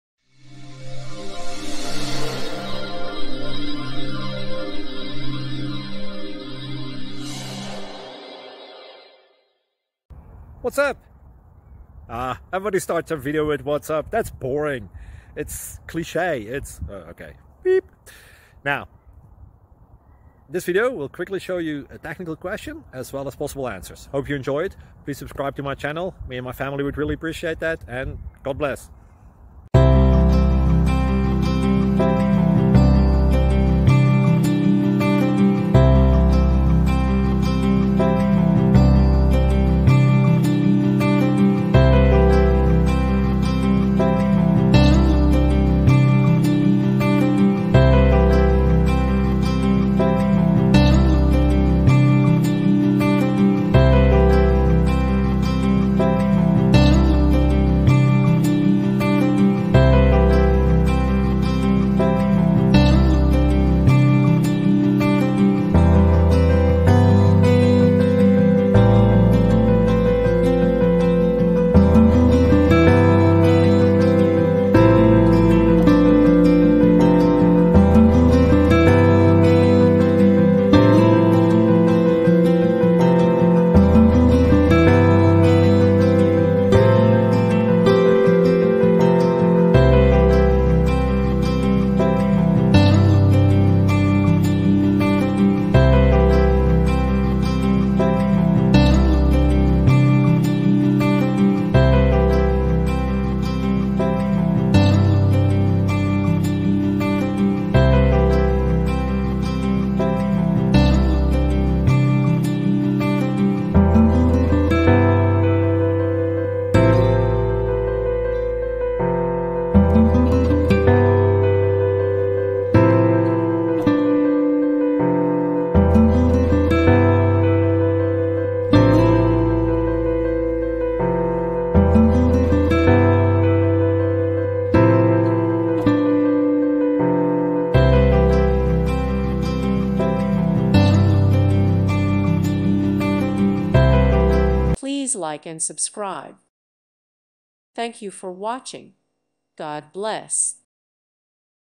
Software Engineering: Implementing a factory design pattern for GA selection
With thanks & praise to God, and with thanks to the many people who have made this project possible! You are responsible for your own actions.